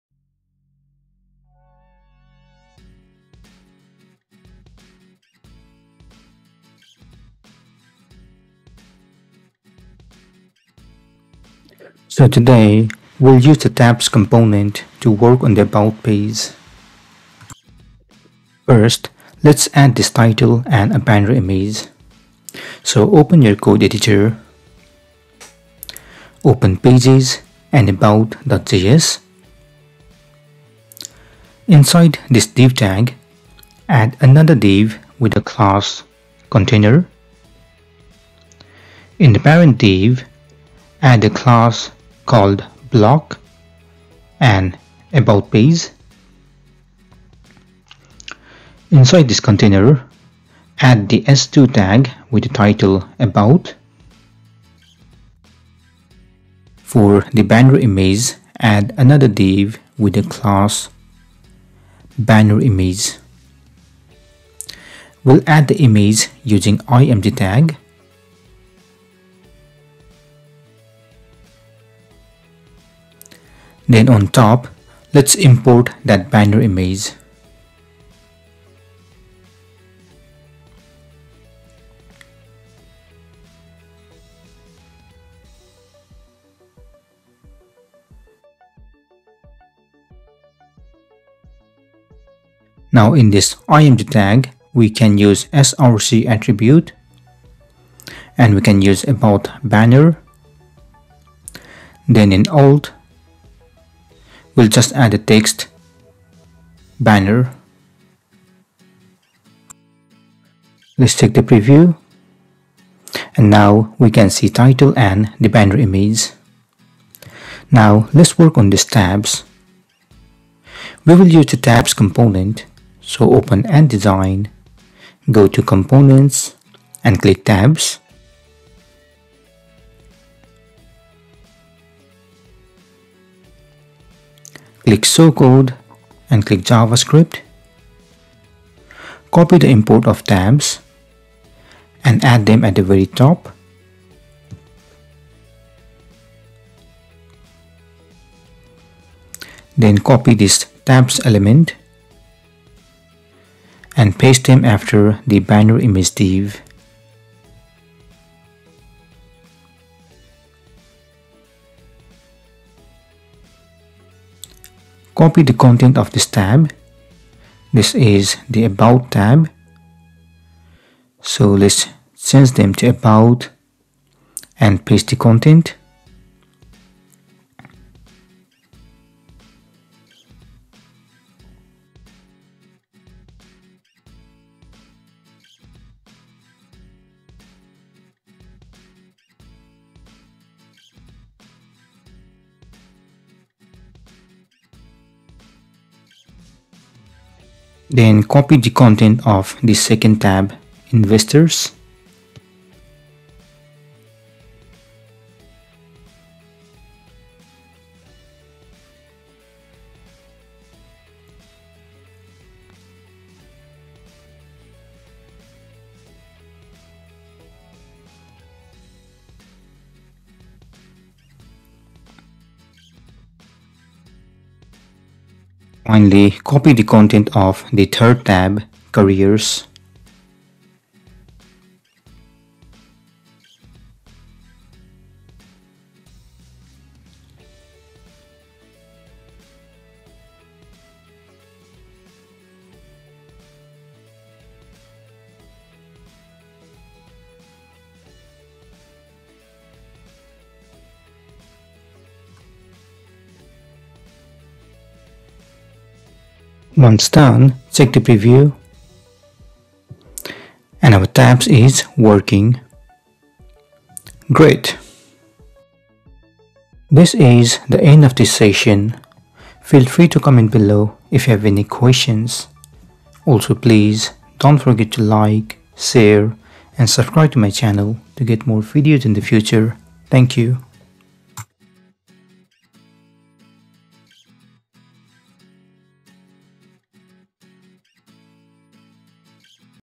React JS #13 - Use tabs component to work on about page
Introduction:
Learn React Ant Design with Real Project.
In this tutorial, I'll show you how to use tabs component to work on about page.